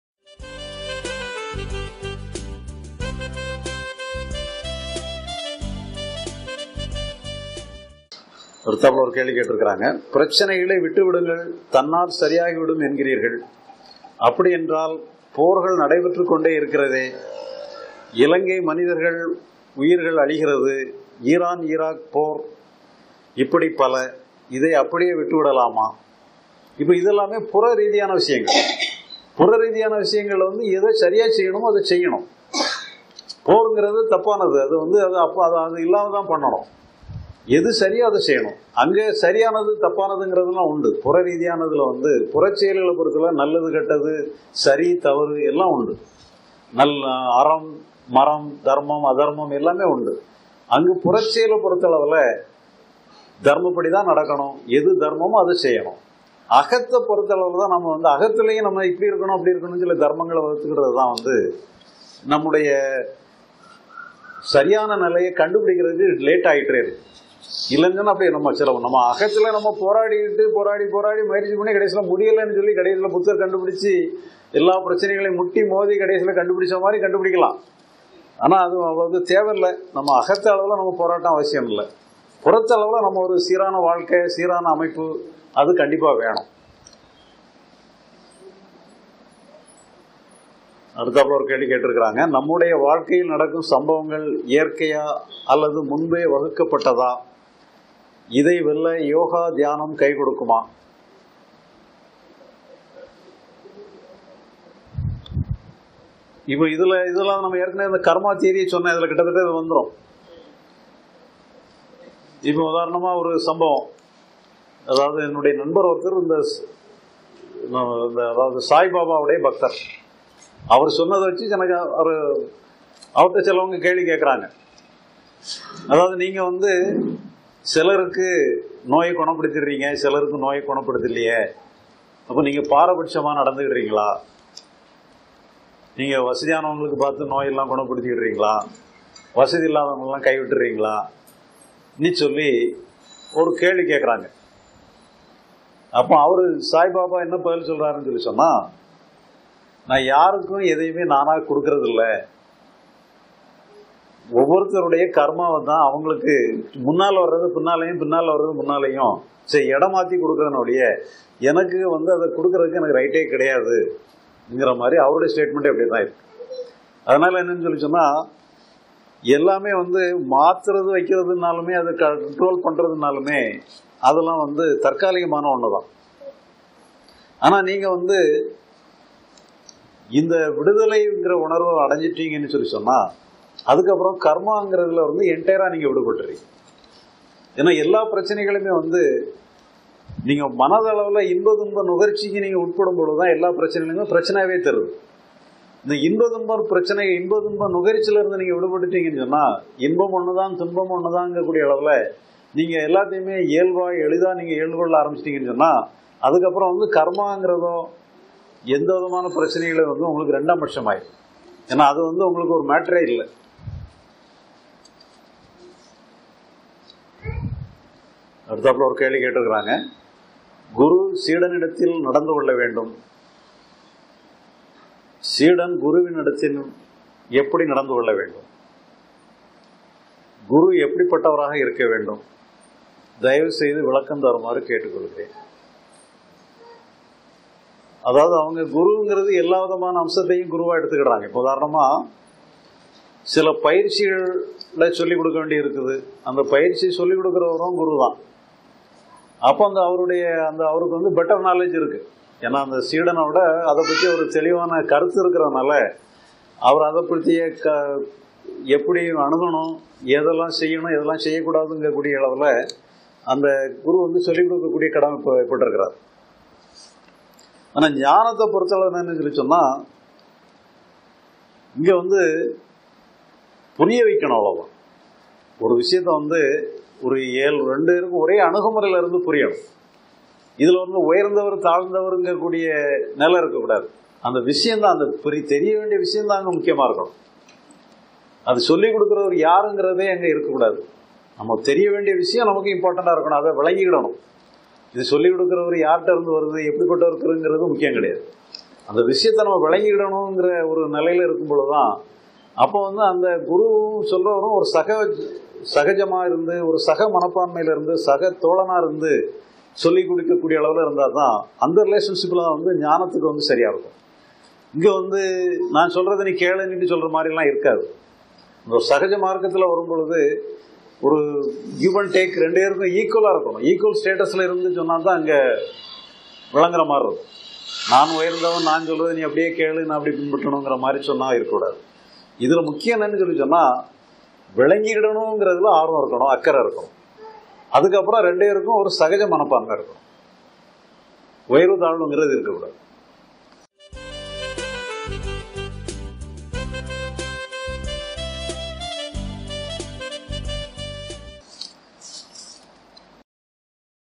1682019 குரு Master மனதை சரியாக வைத்து கொள்வது எப்படி? Sri Bagavath Q&A
Sri பகவத் ஐயா Introduction Whatsapp குழுவில் இணைய நம்முடைய வாழ்க்கையில் நடக்கும் சம்பவங்கள் இயற்கையா ?      குரு எப்படி பட்டவராக இருக்க வேண்டும்?